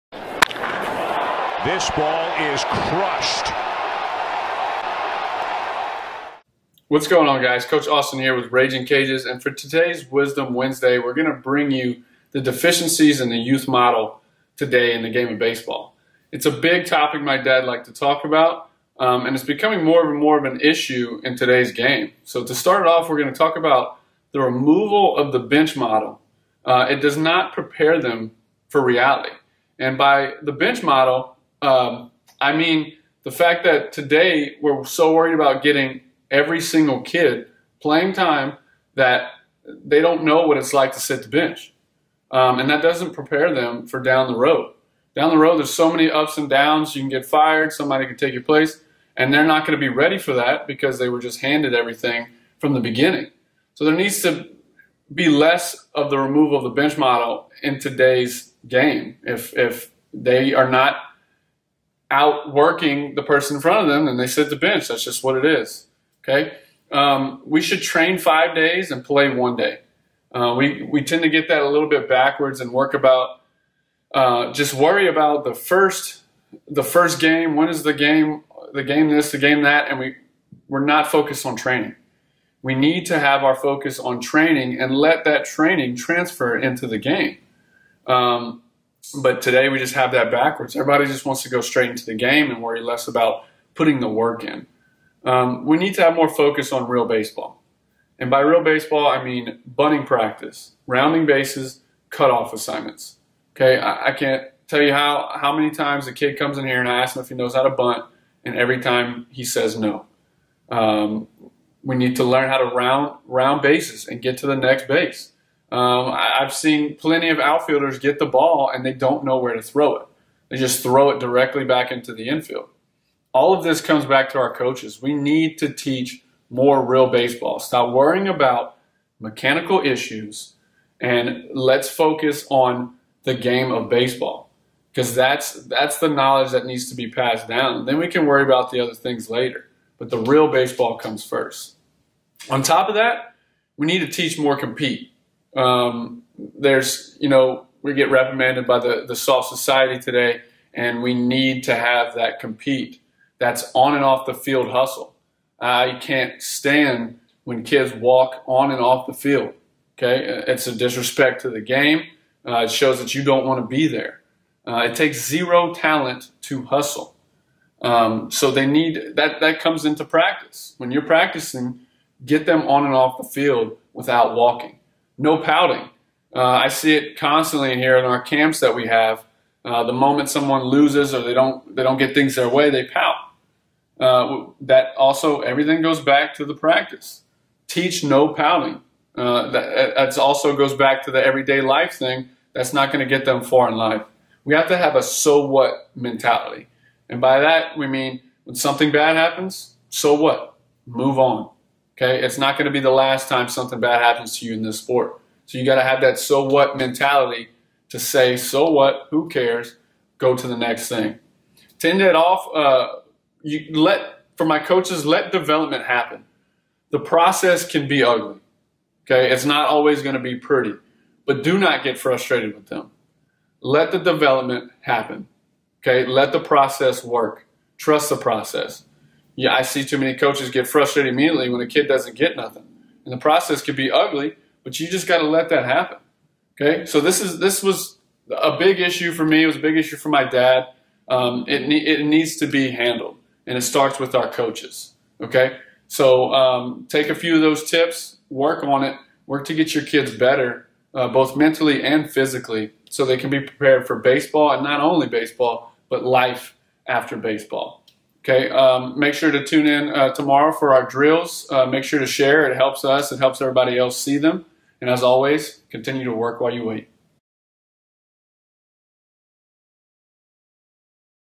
Deficiencies in Youth Model
Coach Austin Robichaux of Ragin Cages - baseball instructional video